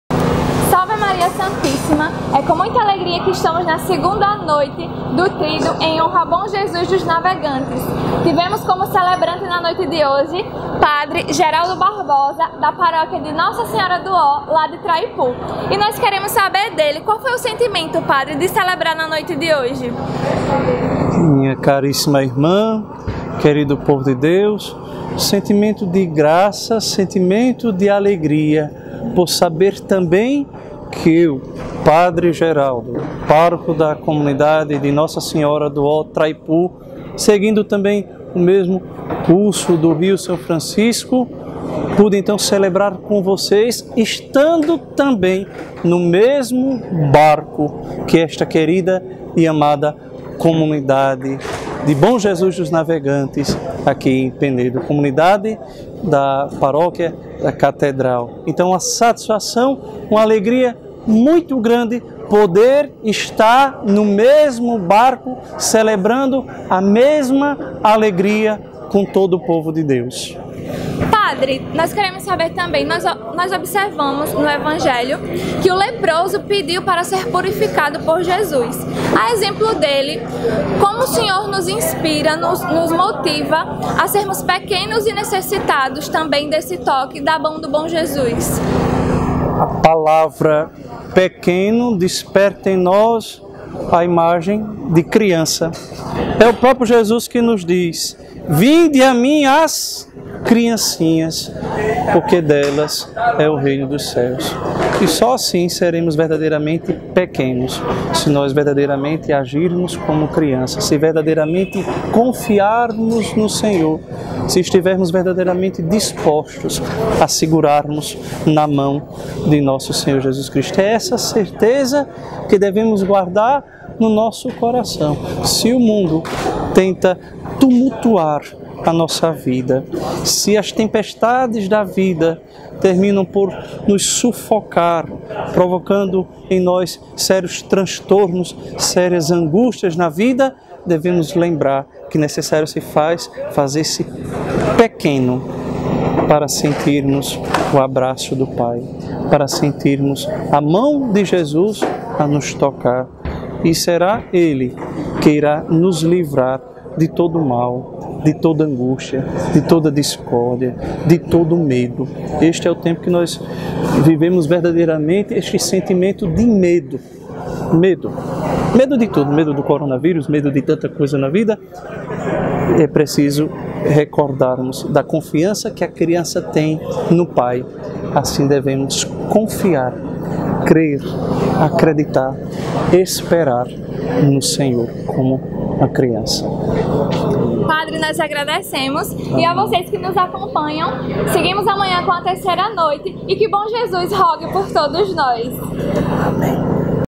ENTREVISTA COM PE. GERALDO BARBOSA | FESTA DO BOM JESUS DOS NAVEGANTES 2021
Entrevista com o padre Geraldo, pároco da paróquia Nossa Senhora do Ó (Traipú/AL), que foi o celebrante na 2ª noite dos festejos ao Bom Jesus dos Navegantes 2021.